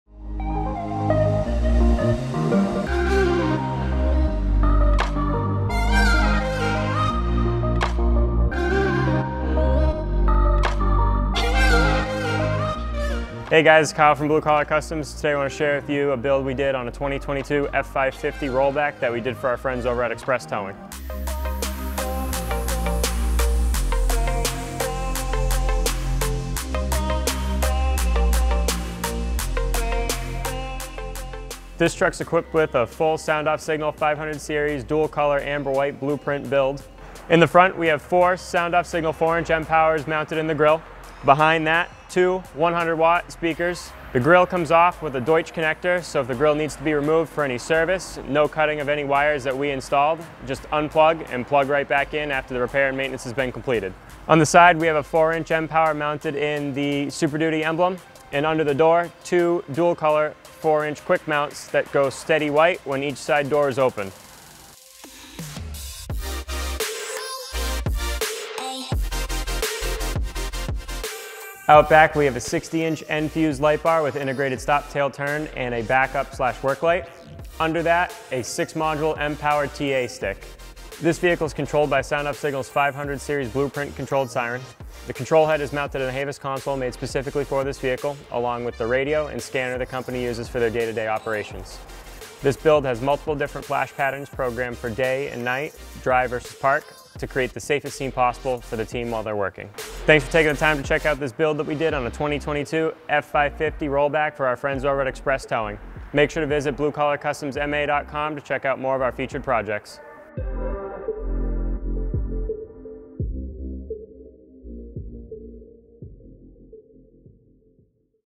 2022 F-550 Rollback | Express Towing | South Shore MA | Blue Collar Customs
Thank you to the Express Towing team that trusted us with their new 2022 F-550 rollback. Check out this build featuring a SoundOff Signal 500 series bluePRINT siren, mpower traffic controller, nFUSE lightbar and more.